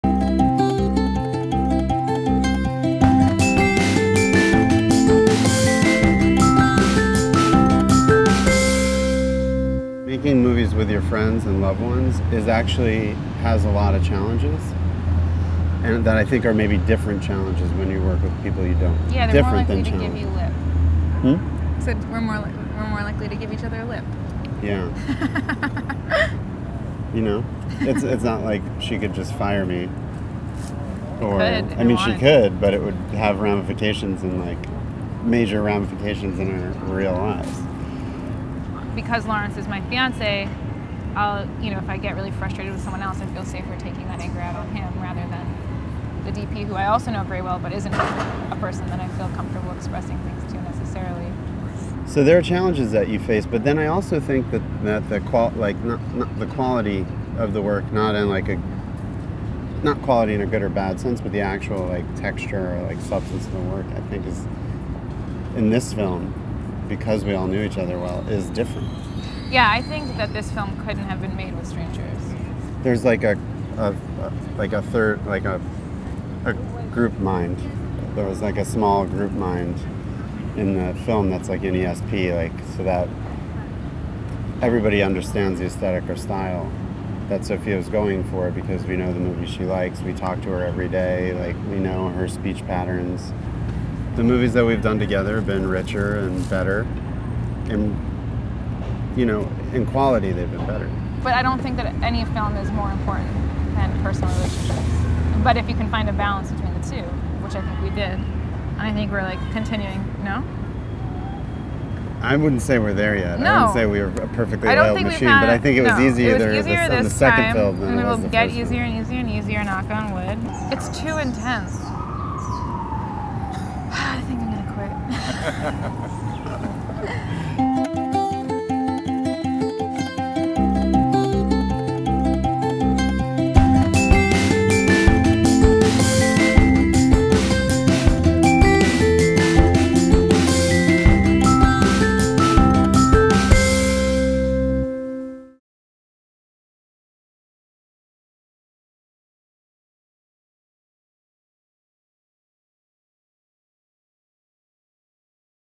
Sophia Takal and Lawrence Michael Levine Buy Some Peas
In the sixth BAMcinemaFest video, filmmaker Sophia Takal and actor Lawrence Michael Levine (of the film GREEN) buy some peas at the Borough Hall Greenmarket. GREEN plays at BAMcinemaFest on Wed, Jun 22 at 9:15pm.